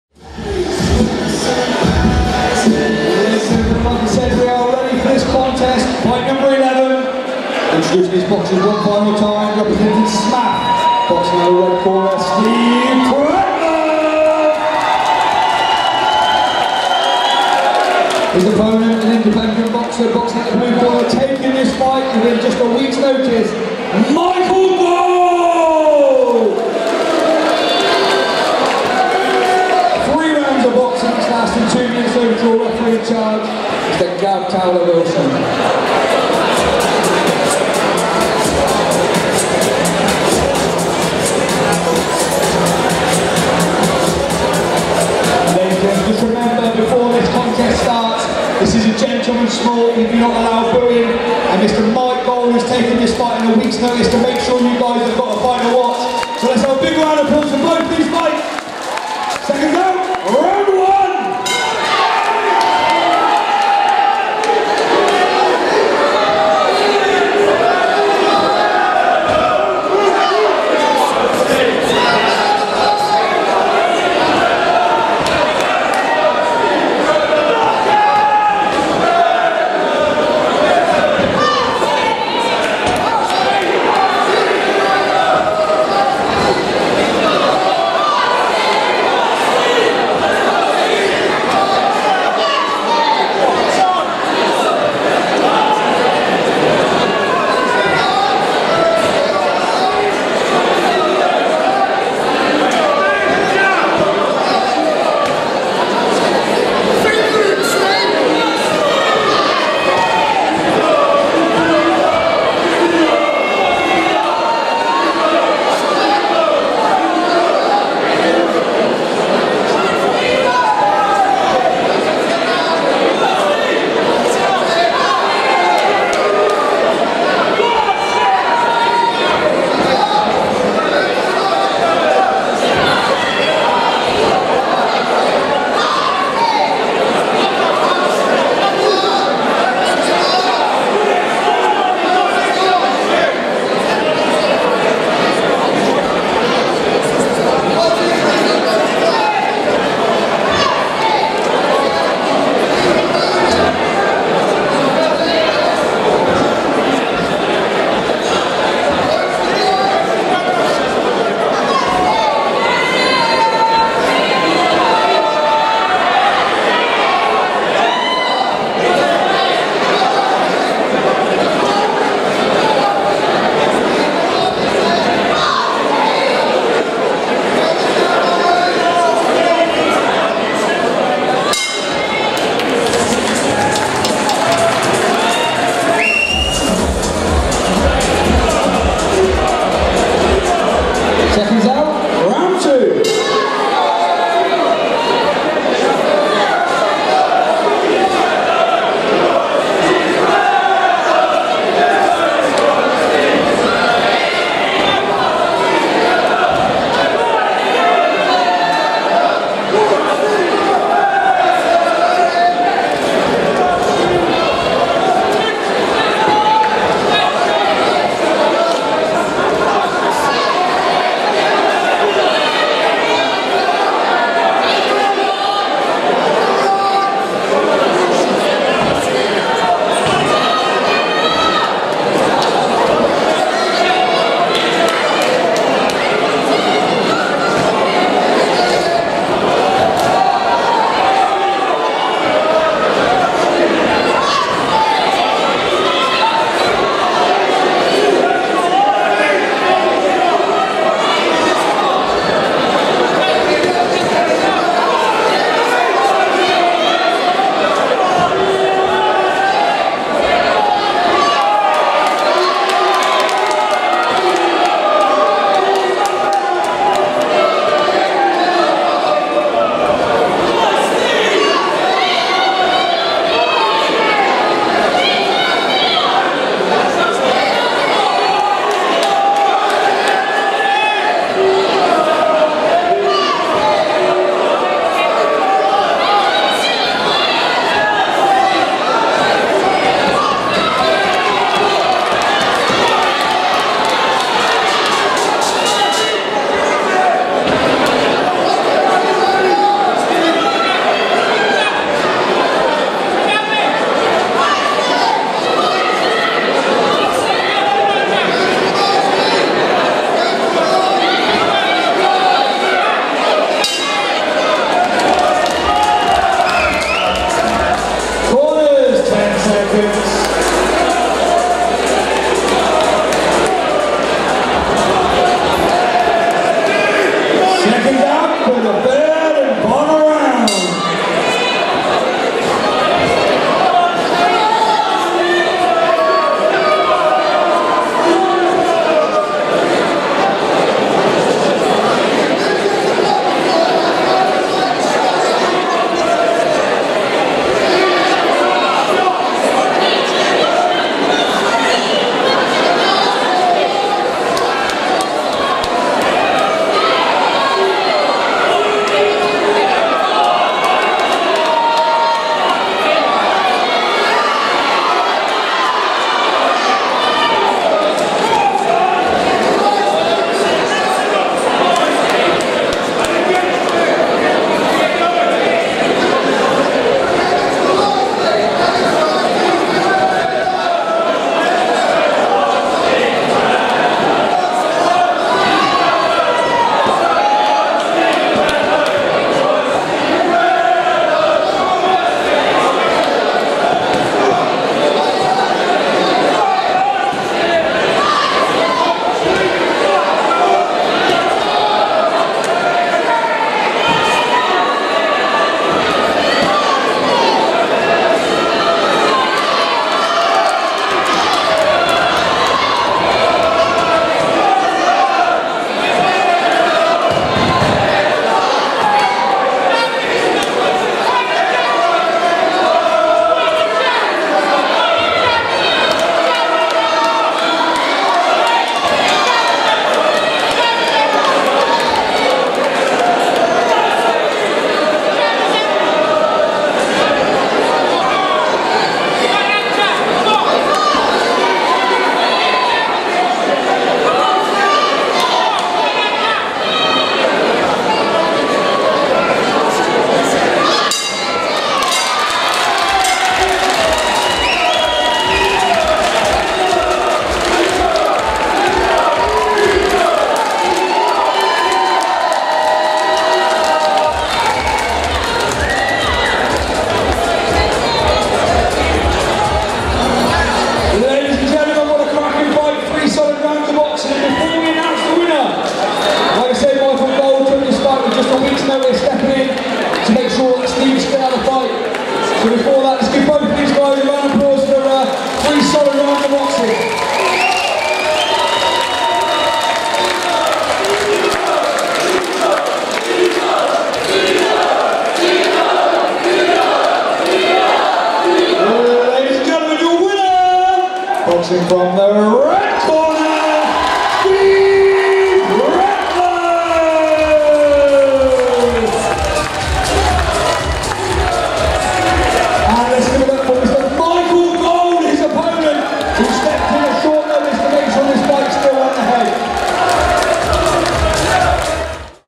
Northern Boys A Night of Champions V Michael Gold Vs Steve Pretlove
Northern Boys Promotions
A Night of Champions V
Arena Vs SMAF
Michael Gold Vs Steve Pretlove
Swindon Meca
23/02/2019

South West Media VIP
First month FREE